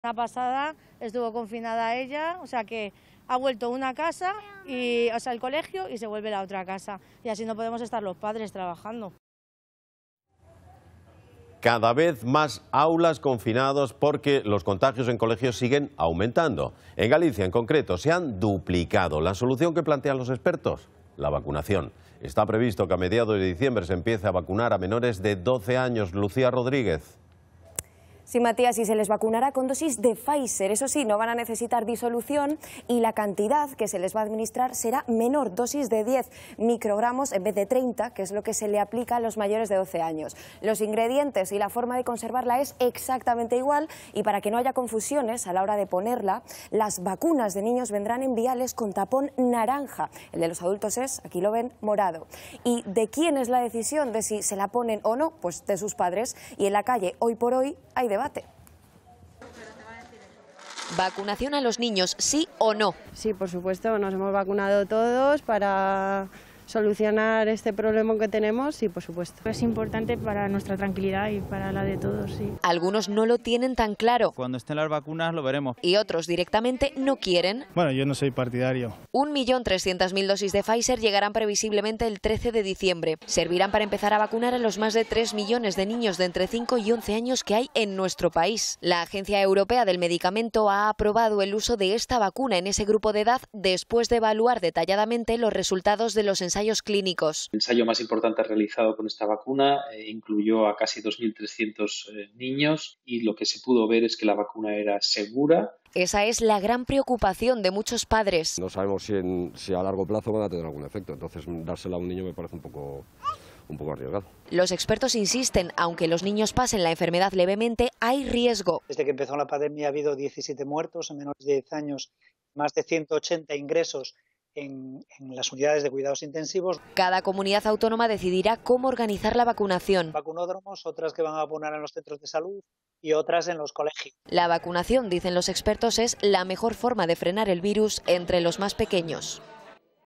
BigDatosA3N | Claves sobre la vacunación a los menores de 12 años
A partir de mitad de diciembre está previsto comenzar a vacunar a menores de 12 años. Estas son algunas de las claves.